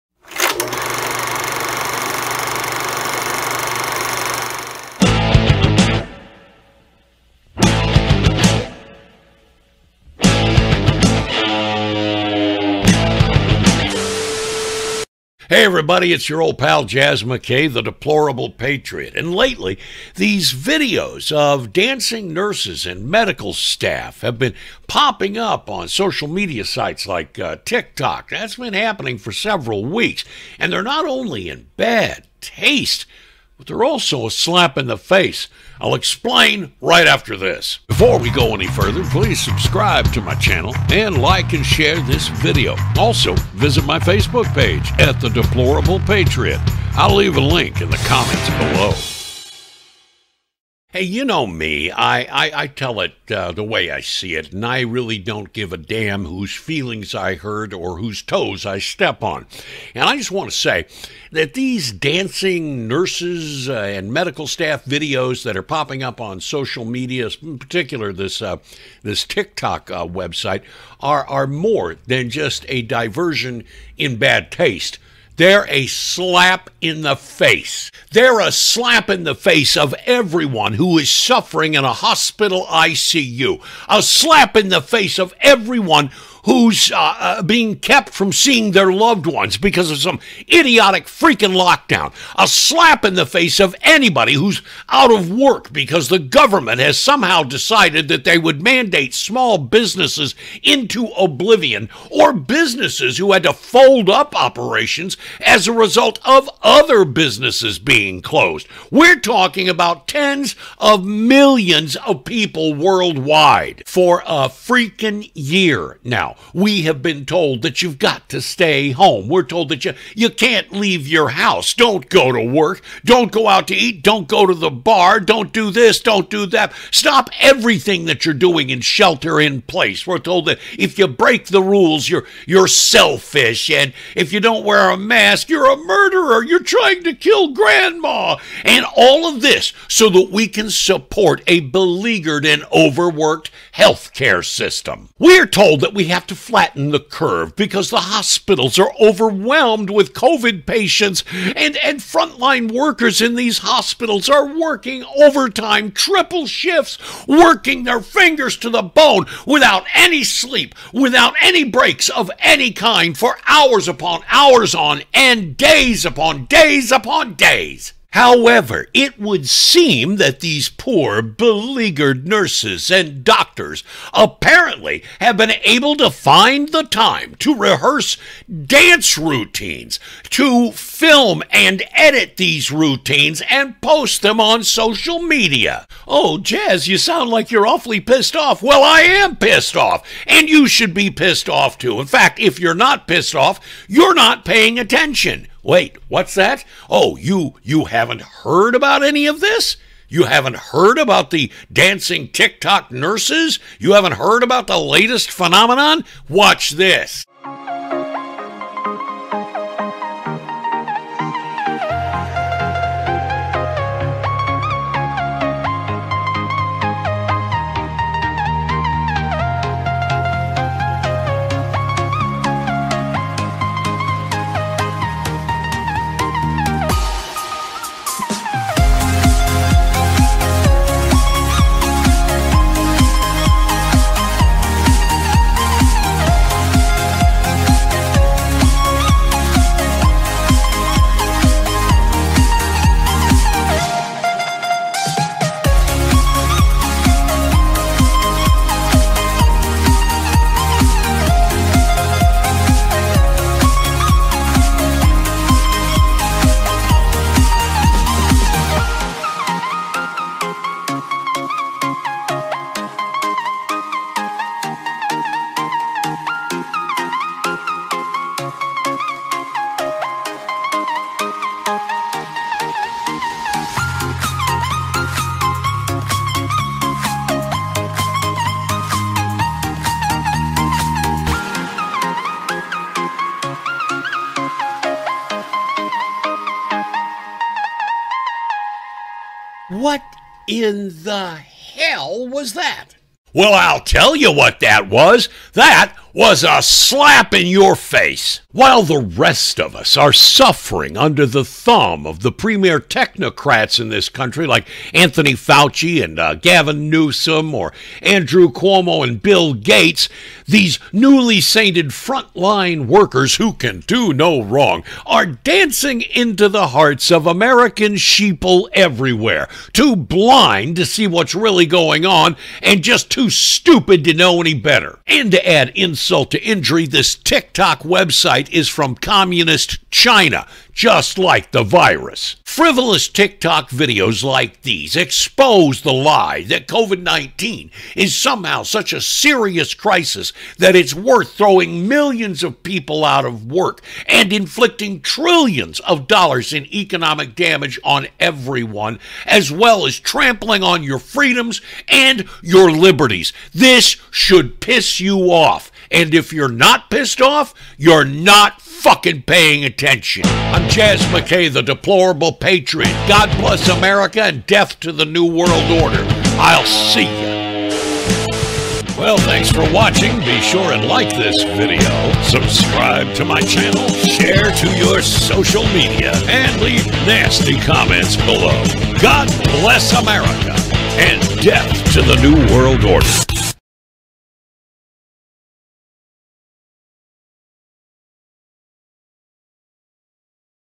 The Dance Of The TikTok Nurses
Videos of dancing nurses have been circulating on TikTok for several weeks now and they aren’t just in bad taste.

They are a slap in the face.

These frivolous TikTok videos expose the lie that COVID-19 is such a serious crisis that it justifies throwing millions out of work, inflicting $trillions in economic damage, and suppressing core freedom and liberties.

Unlike the Lame Stream Media I am NOT funded by Bill & Melinda Gates, or George Soros, and this channel is not monetized, so a few coins in my jar to help me keep going is greatly appreciated.